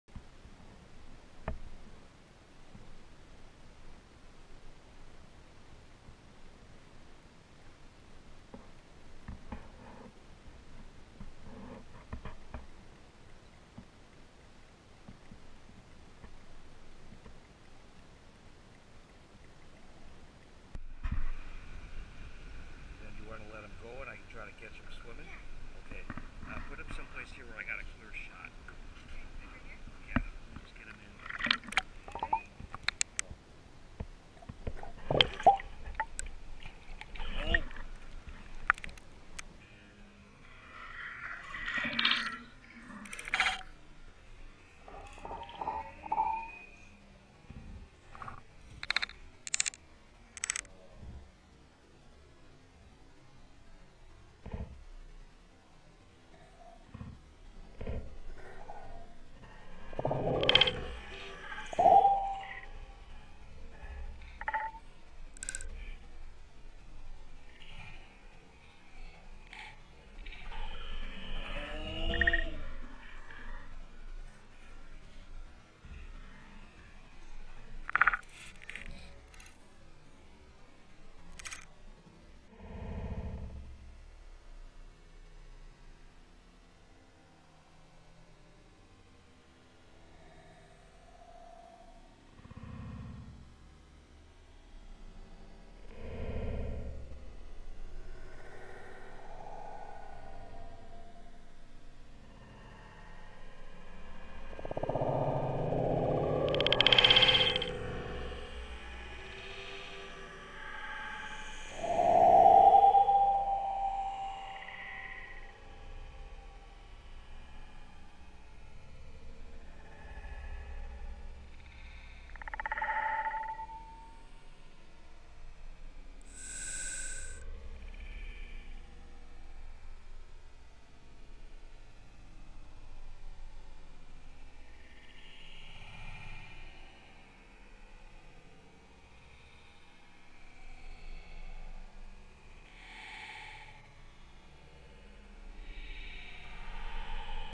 Bass Eats Frog
Part of the Riveredge Trip Hacks series, a series of demonstration "shorts" and quick nature captures to prepare you for your visit to Riveredge.

Video taken in Farm Pond. Unscripted frog release shows bass lingering in the "weeds" awaiting his next meal. Wait for super slow motion at ~2 minutes.

Courtesy of Scott Nehring.